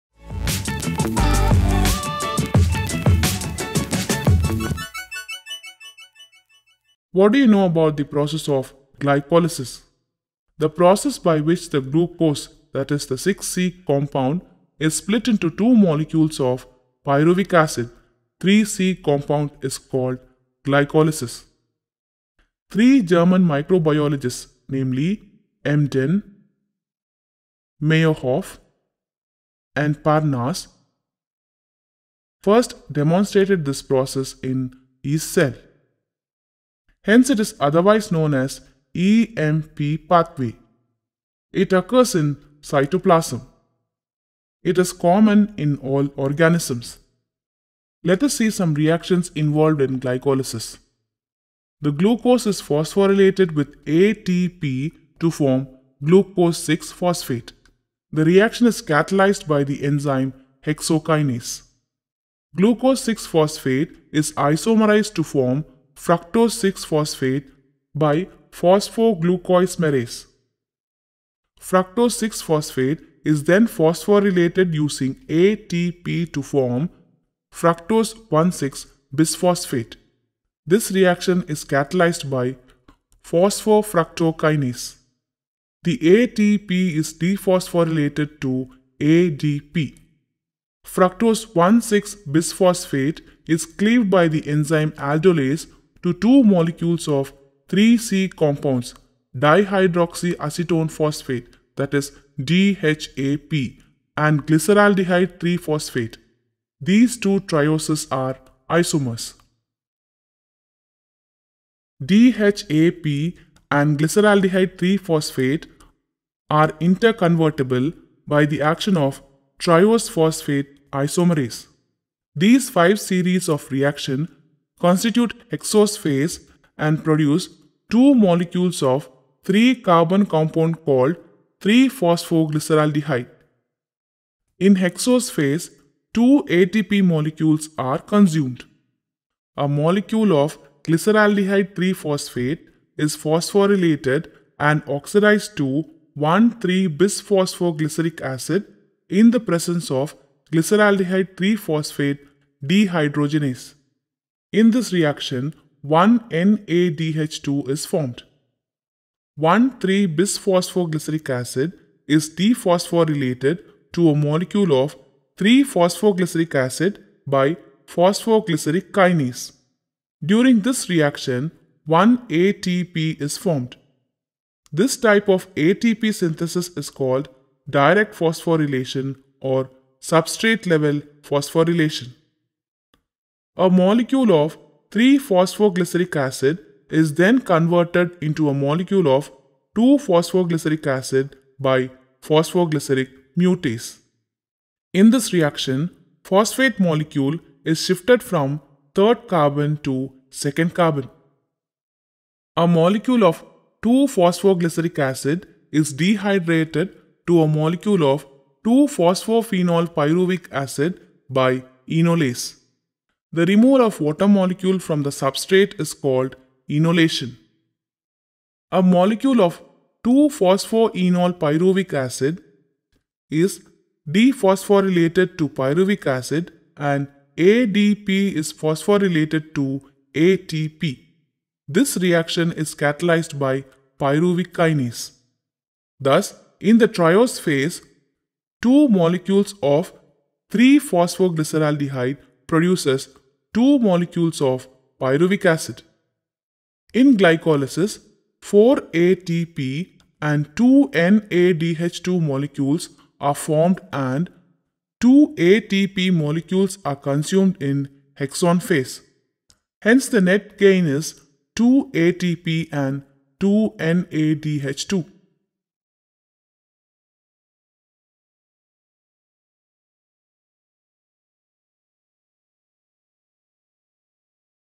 GLYCOLYSIS PATHWAY | REACTIONS IN GLYCOLYSIS | EMP PATHWAY BIOCHEMISTRY
The process by which the glucose (6C compound) is split into two molecules of pyruvic acid (3C compound) is called glycolysis. Three German Microbiologists – Embden, Meyerhof and Parnas, first demonstrated this process in yeast cell. Hence, it is otherwise known as EMP pathway. It occurs in cytoplasm. It is common in all organisms. It is divided into two phases – hexose phase and triose phase. Glyceraldehyde 3-phosphate and DHAP are the products of hexose phase and two molecules of pyruvic acid are the products of triose phase. The overall reaction of glycolysis is given in the following equation.
Reactions involved in glycolysis are as follows:
1. The glucose is phosphorylated with ATP to form glucose-6-phosphate. The reaction is catalyzed by the enzyme hexokinase.
2. Glucose-6-phosphate is isomerized to form fructose-6-phosphate by phosphoglucoisomerase.
3. Fructose-6-phosphate is then phosphorylated using ATP to form fructose 1,6-bisphosphate. This reaction is catalyzed by phosphofructokinase. The ATP is dephosphorylated to ADP.
4. Fructose 1,6-bisphosphate is cleaved by the enzyme aldolase to two molecules of 3C compounds – dihydroxy acetone phosphate (DHAP) and glyceraldehyde 3-phosphate. These two trioses are isomers.
5. DHAP and glyceraldehyde-3-phosphate are interconvertible by the action of triose phosphate isomerase. These five series of reaction constitute hexose phase and produce two molecules of 3-carbon compound called 3- phosphoglyceraldehyde. In hexose phase two ATP molecules are consumed.
6. A molecule of glyceraldehyde-3-phosphate is phosphorylated and oxidized to 1,3-bisphosphoglyceric acid in the presence of glyceraldehyde-3-phosphate dehydrogenase. During this reaction, one NADH2 is formed.
7. 1,3-bisphosphoglyceric acid is dephosphorylated to a molecule of 3-phosphoglyceric acid by phosphoglyceric kinase. During this reaction one ATP is formed. This type of ATP synthesis is called direct phosphorylation or substrate level phosphorylation.
8. A molecule of 3-phosphoglyceric acid is then converted into a molecule of 2-phosphoglyceric acid by phosphoglyceric mutase. In this reaction, phosphate molecule is shifted form third carbon to second carbon.
9. A molecule of 2-phosphoglyceric acid is dehydrated to a molecule of 2-phosphoenol pyruvic acid by enolase. Removal of water molecule from the substrate is called enolation.
10. A molecule of 2-phosphoenol pyruvic acid is dephosphorylated to pyruvic acid and ADP is phosphorylated to ATP. This reaction is catalyzed by pyruvic kinase. Thus, in the triose phase, two molecules of a molecule of 3-phospho glyceraldehyde produce 2 molecules of pyruvic acid. In glycolysis, 4ATP and 2NADH2 molecules are formed and 2ATP molecules are consumed in hexose phase. Hence, the net gain is 2ATP and 2NADH2.